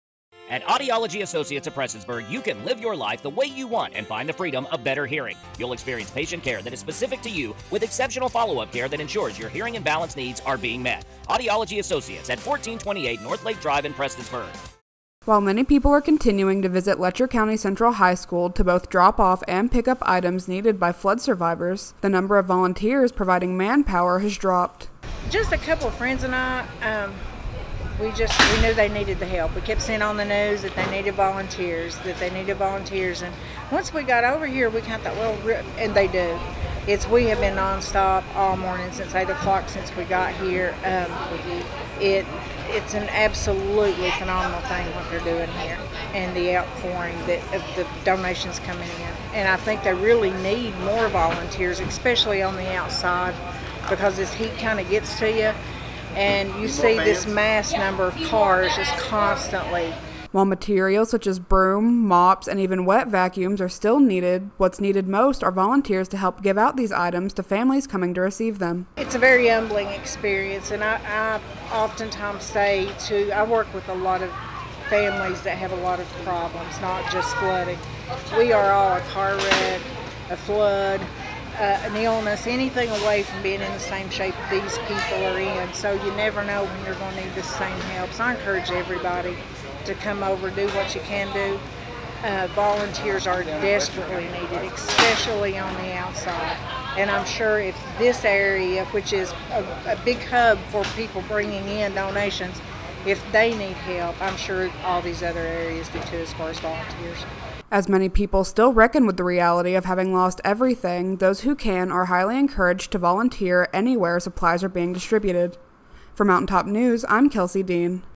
Letcher County Central High School In Need of Volunteers
While donations are still going strong, Letcher County Central High School is in need of volunteers to help flood victims.